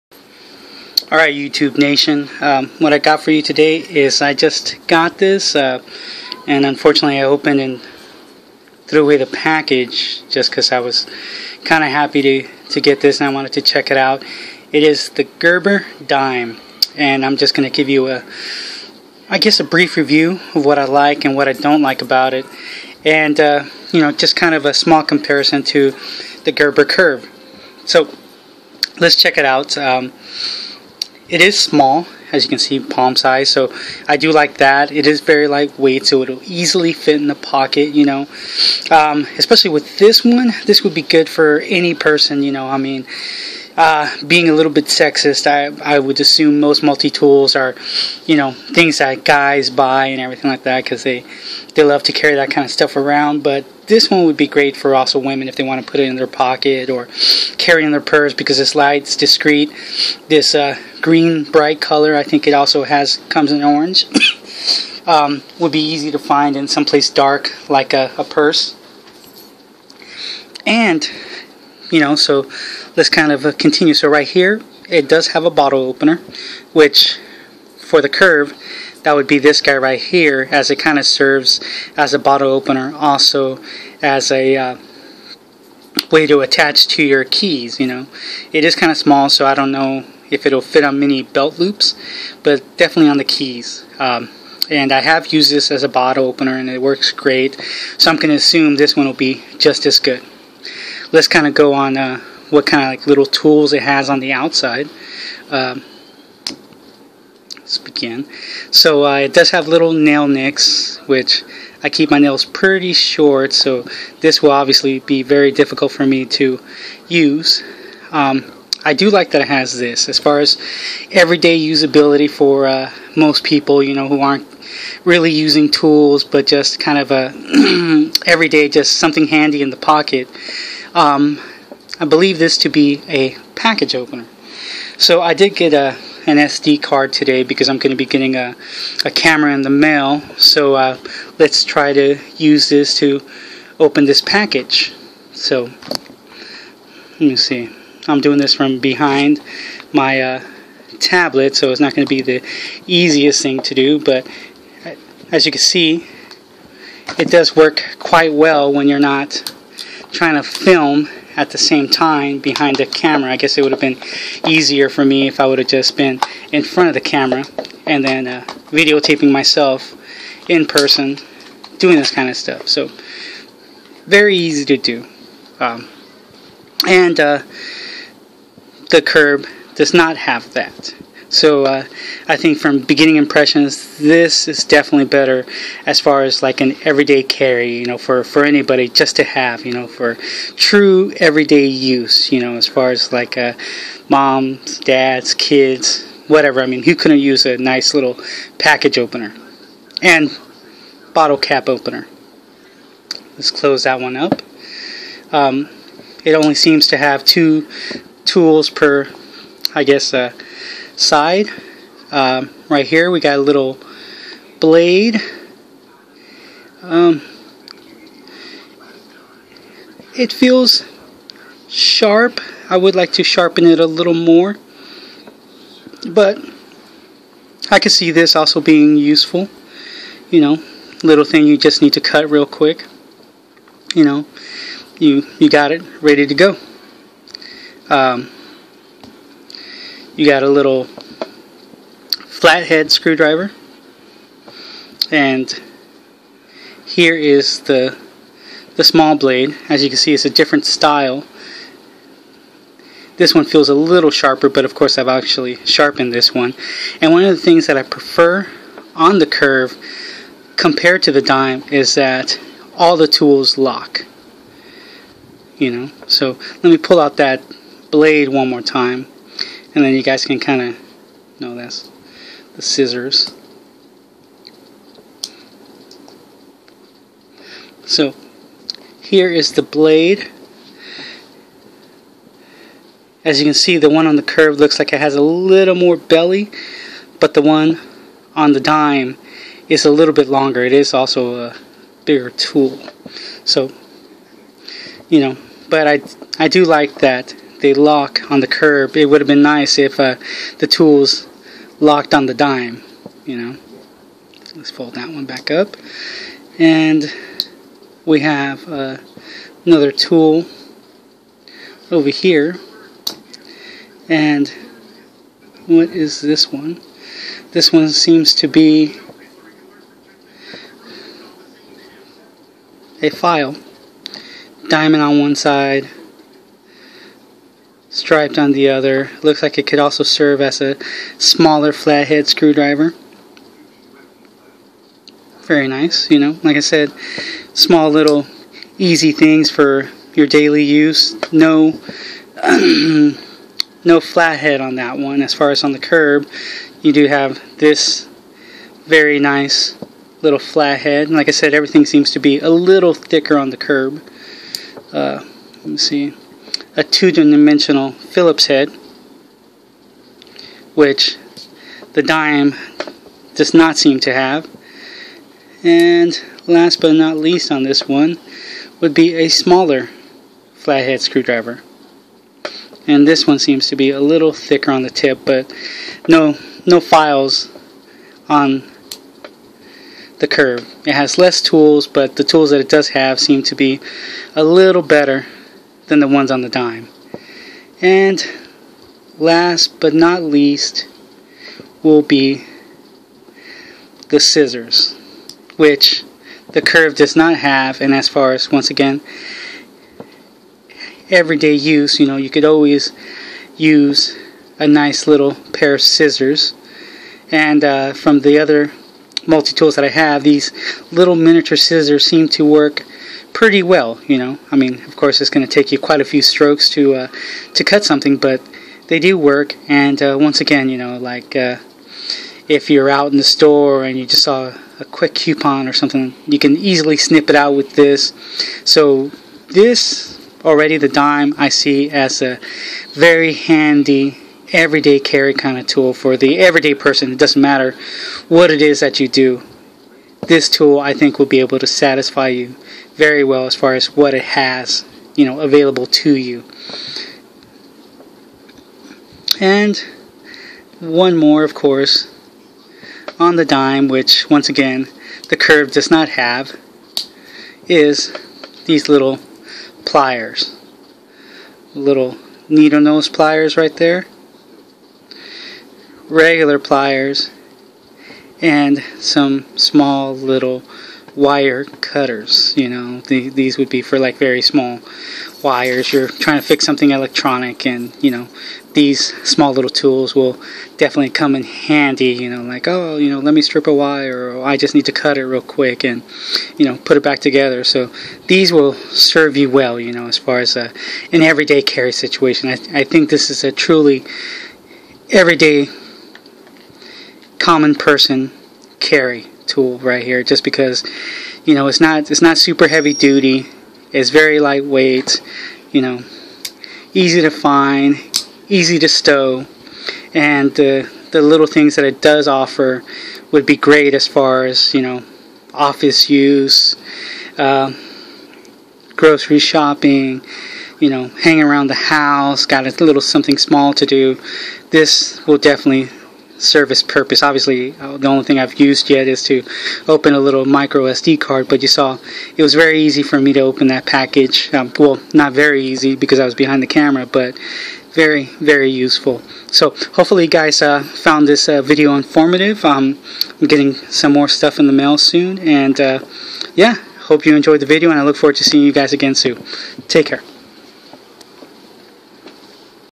Gerber Dime multitool review and comparison too th
This is a review of the Gerber Dime multitool,  this is an awesome everyday carry tool and I will also compare it to the Gerber Dime.